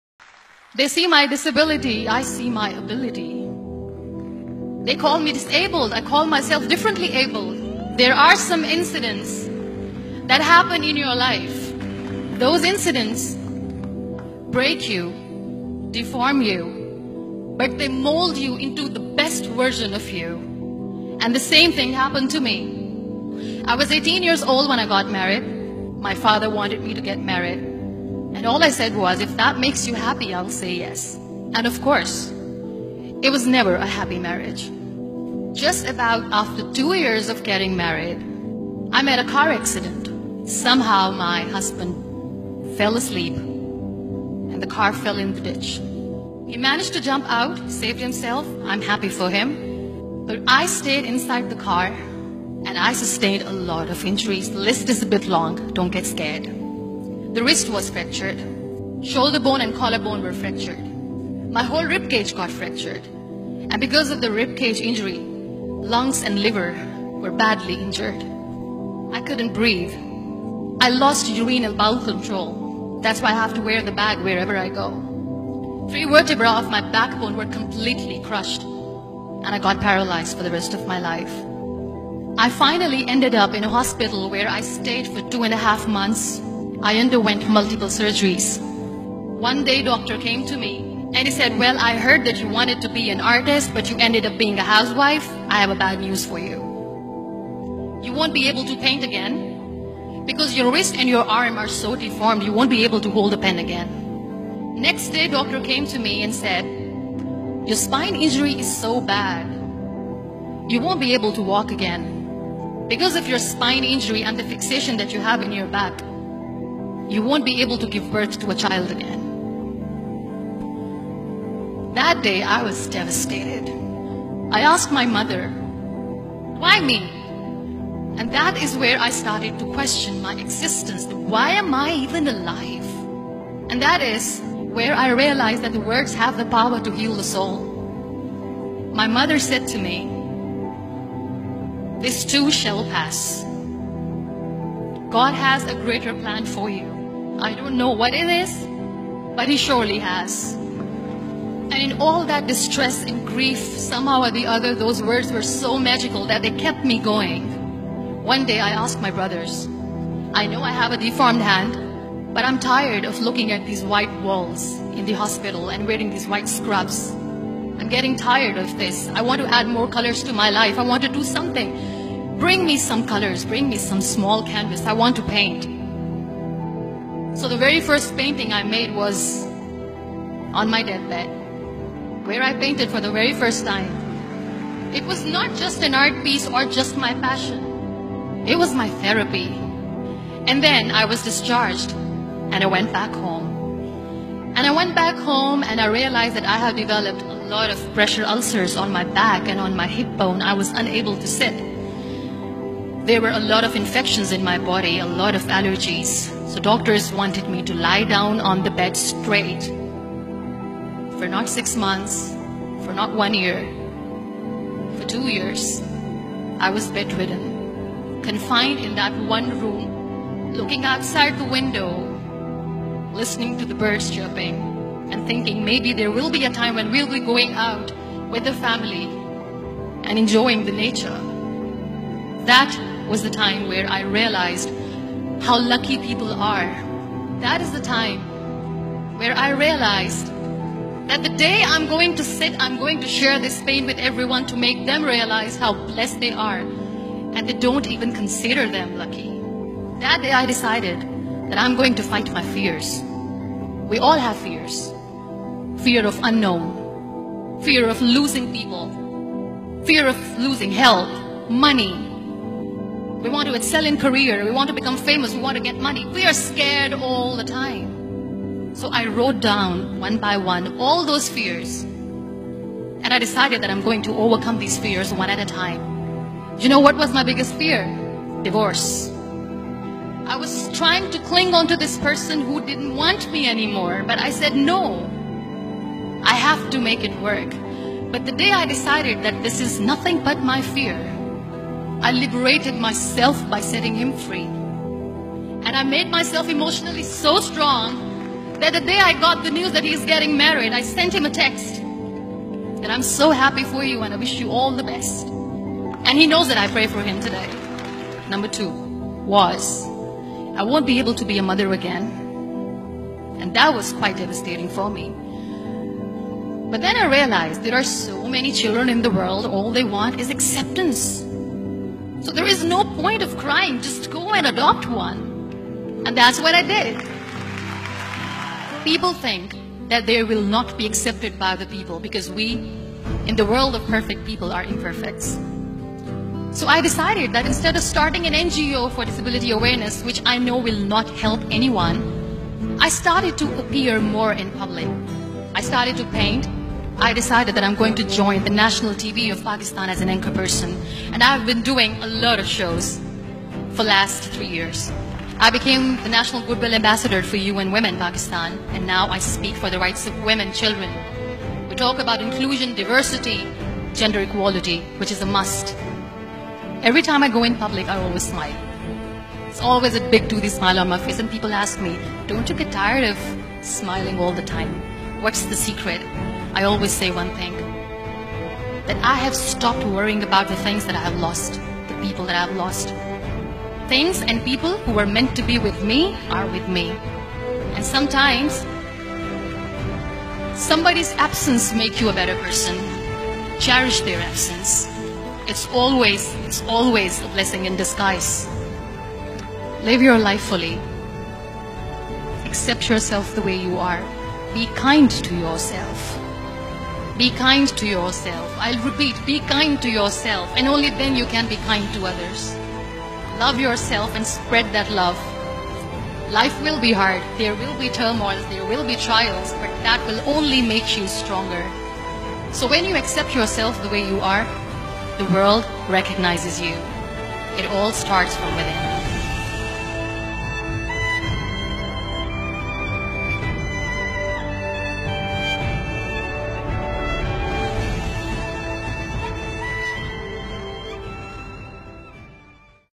Motivational Speech by Muniba Mazari
Muniba Mazari Baloch (Urdu: منیبہ مزاری; born 3 March 1987, also known as the Iron Lady of Pakistan) is a Pakistani activist, anchor artist, model, singer and motivational speaker. She became the National Ambassador for UN Women Pakistan after being shortlisted in the 100 Inspirational Women of 2015 by BBC. She also made it to the Forbes 30 under 30 list for 2016.

Muniba Baloch is also Pakistan's first model and anchor who uses a wheelchair. She uses a wheelchair due to injuries sustained in a car crash at the age of 21. She appeared as a host on Hum News's social show Main Nahi Hum.

Source : wikipedia

National UN Women Ambassador Muniba Mazari